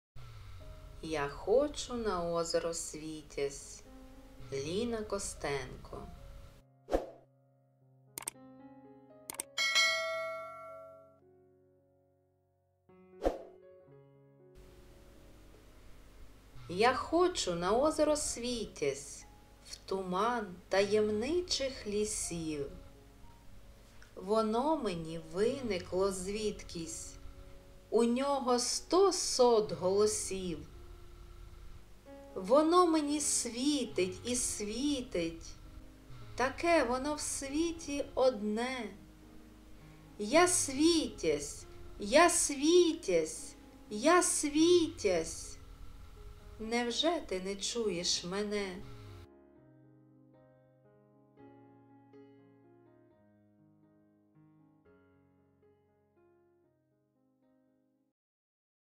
Я хочу на озеро Світязь (за Савченко, 4 кл.) – Ліна Костенко
Пропоную для вашої уваги це відео із поетичним твором українського автора Ліни Костенко "Я хочу на озеро Світязь". Бажаю приємного перегляду та задоволення від прослуховування.
Предлагаю вашему вниманию это видео с поэтическим произведением украинского автора Лины Костенко "Я хочу на озеро Свитязь".

Я хочу на озеро Світязь,
в туман таємничних лісів.
Воно мені виникло звідкись,
у нього сто сот голосів.
Воно мені світить і світить,
таке воно в світі одне.
— Я Світязь! Я Світязь! Я Світязь!
Невже ти не чуєш мене?!